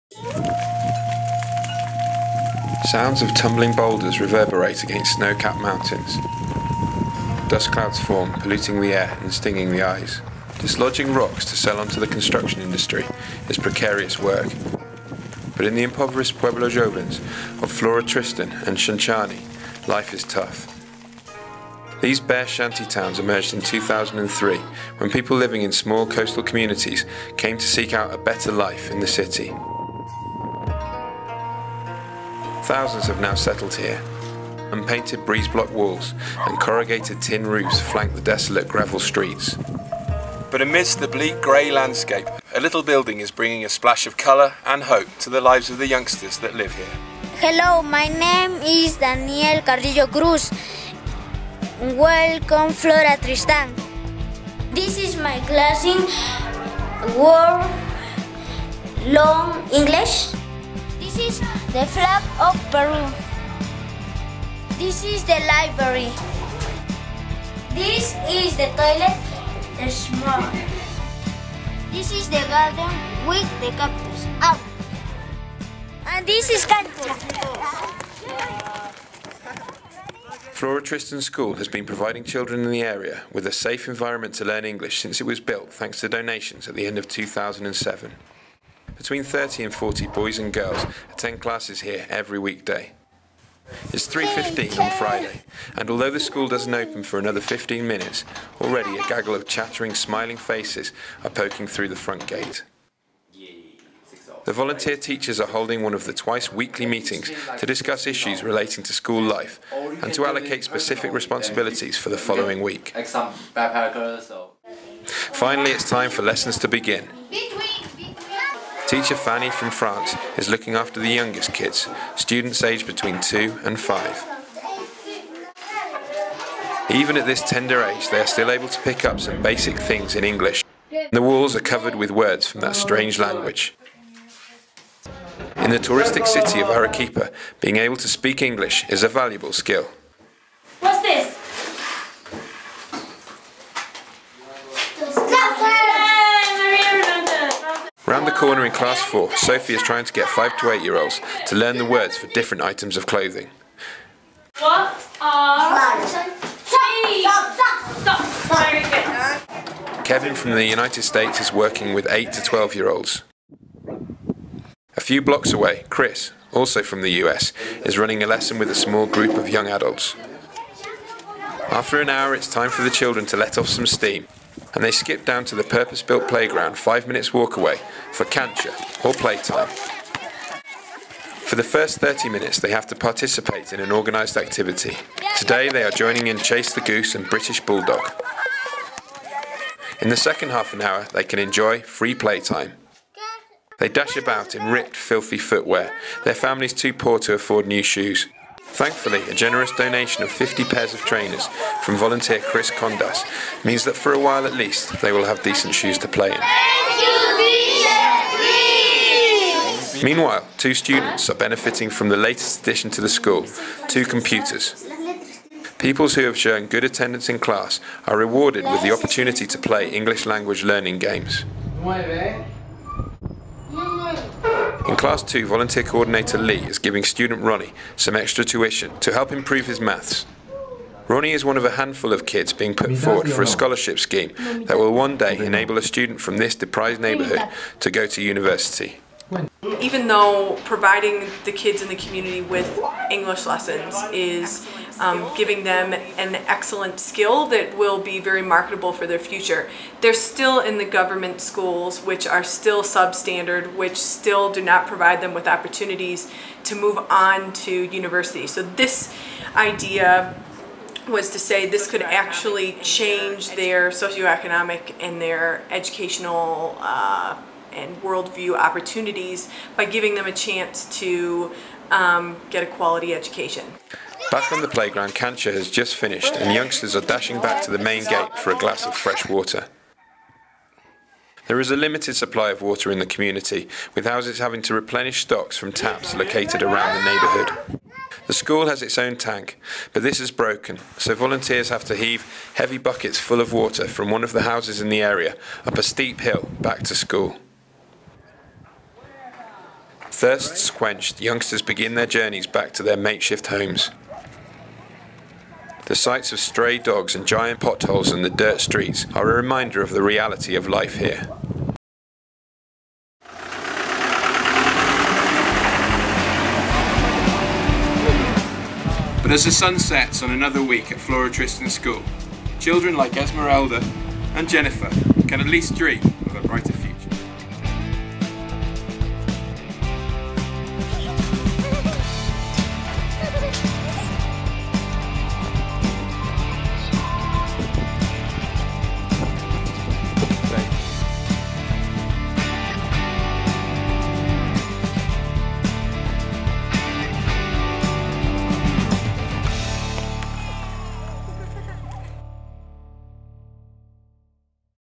Flora Tristan English School.mov
A school run by volunteers in a disadvantaged area of Arequipa, Peru.
Video taken in Autumn, 2011.